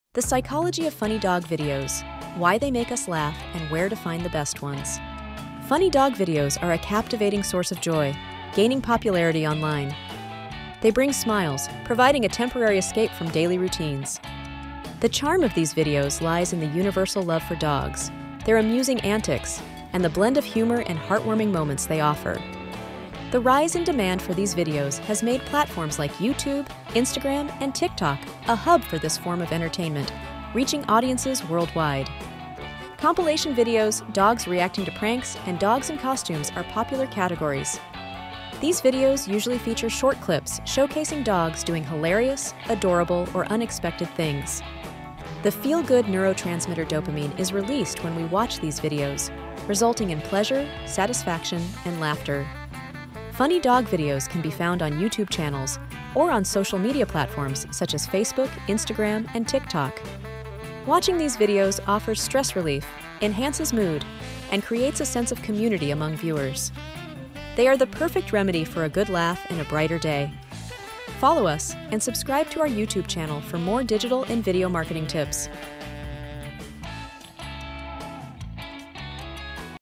Why Do People Like Funny Dog Videos?
Why They Make Us Laugh and Where to Find the Best Ones.

Funny dog videos are a captivating source of joy, gaining popularity online.

They bring smiles, providing a temporary escape from daily routines.

These videos' charm lies in their universal love for dogs, their amusing antics, and the blend of humor and heartwarming moments they offer.

The rise in demand for these videos has made platforms like YouTube, Instagram, and TikTok a hub for this form of entertainment, reaching audiences worldwide.

Popular categories include compilation videos, dogs reacting to pranks, and dogs in costumes.

These videos usually feature short clips showcasing dogs doing hilarious, adorable, or unexpected things.

The "feel-good" neurotransmitter dopamine is released when we watch these videos, resulting in pleasure, satisfaction, and laughter.

Funny dog videos can be found on YouTube channels or on social media platforms such as Facebook, Instagram, and TikTok.

Watching these videos offers stress relief, enhances mood, and creates a sense of community among viewers.

They are the perfect remedy for a good laugh and a brighter day.